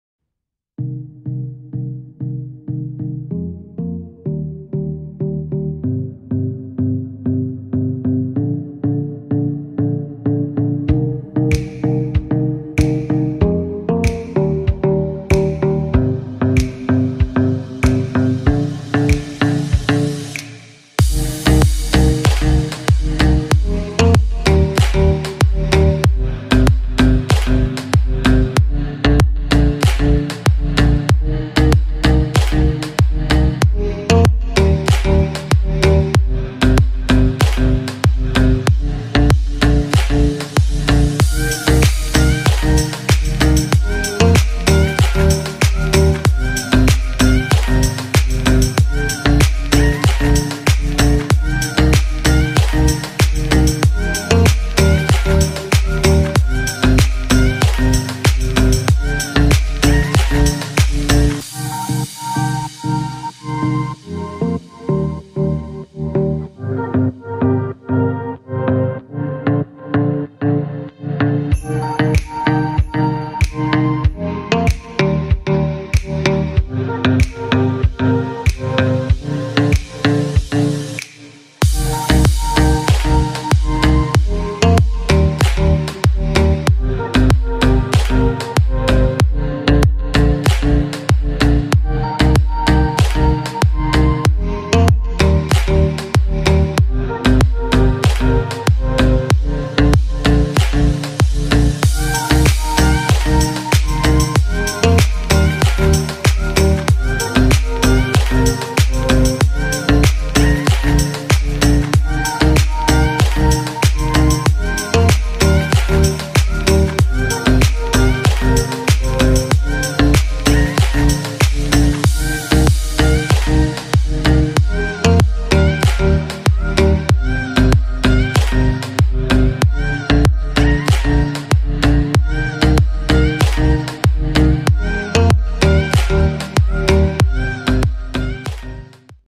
Киев Русановская набережная Фонтаны 4к 2020
Киев. Фонтаны на Русановской набережной. Июнь 2020. Снято на Samsung Galaxy S10 plus в 4к.